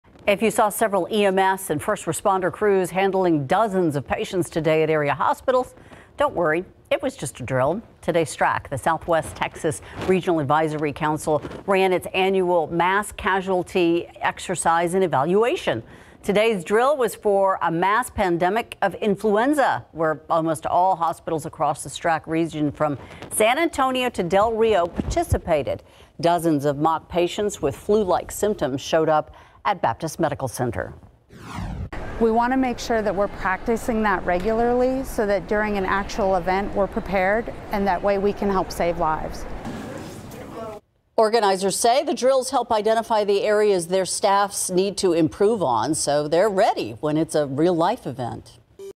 San Antonio-area hospitals, EMS hold region-wide mass casualty drill
On Wednesday, the Southwest Texas Regional Advisory Council ran its annual mass casualty exercise and evaluation.